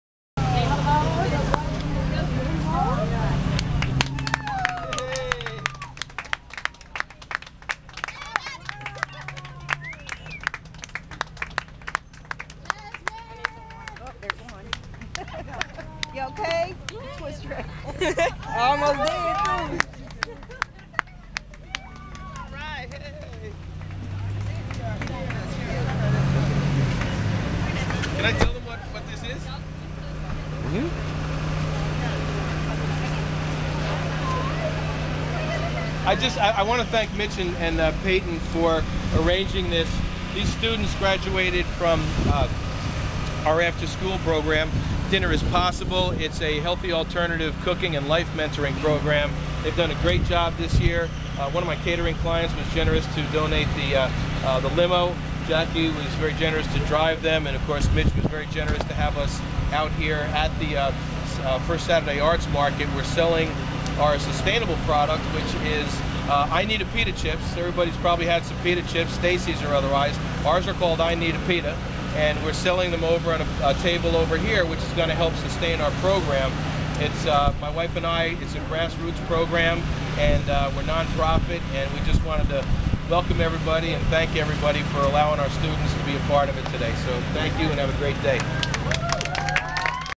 Dinner: IS Possible
Dinner: IS Possible was founded by Chef Tim Mehne in 2007 as a healthy-alternative cooking program. It quickly grew to include a life mentoring program helping youth with the challenges of growing up. I Needa Pita Chips are a healthy-alternative snack that are produced by the youth in Dinner: IS Possible, which raises money to keep the program growing. Read more on the blog.